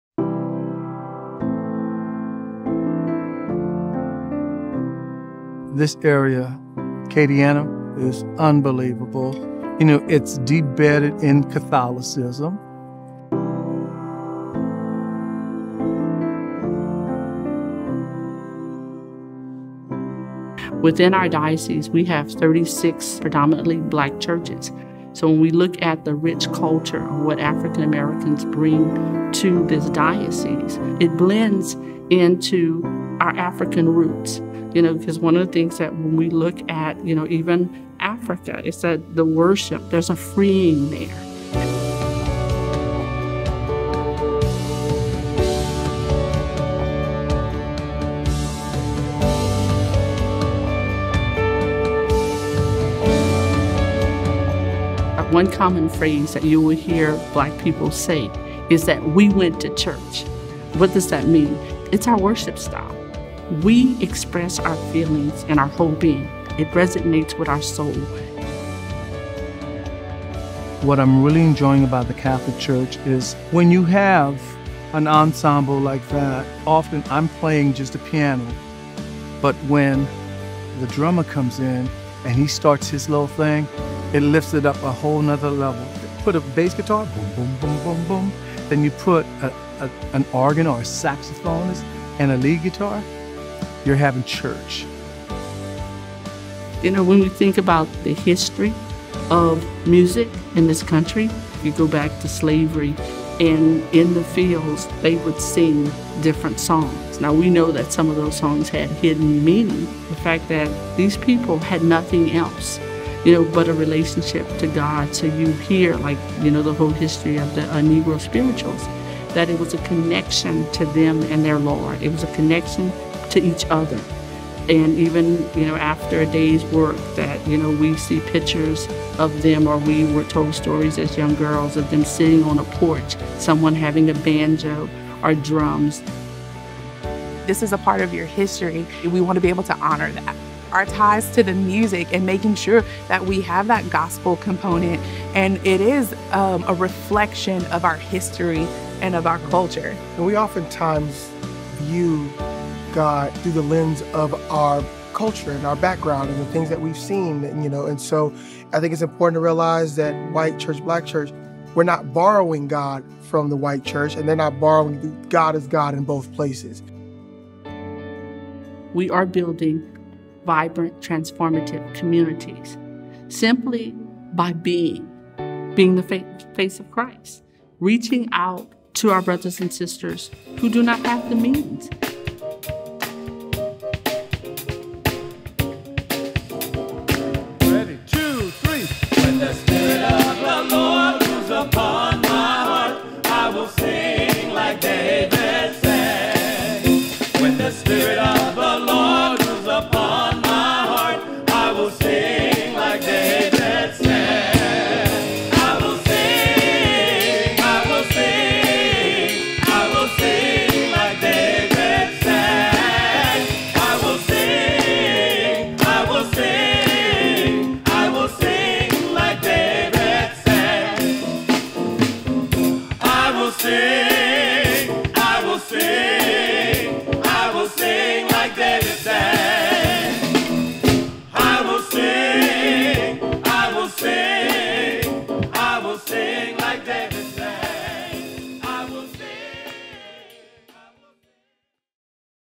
Faith & Culture: Black Catholics in Lafayette
We travel to the Diocese of Lafayette, Louisiana, for the next video in our Faith & Culture series focusing on music in the Black Catholic Church. Within the diocese, there are 36 predominantly Black churches, and for 100 years, we have proudly supported each one of these parishes. Watch how music impacts worship in these Catholic communities that are alive with faith and spirit.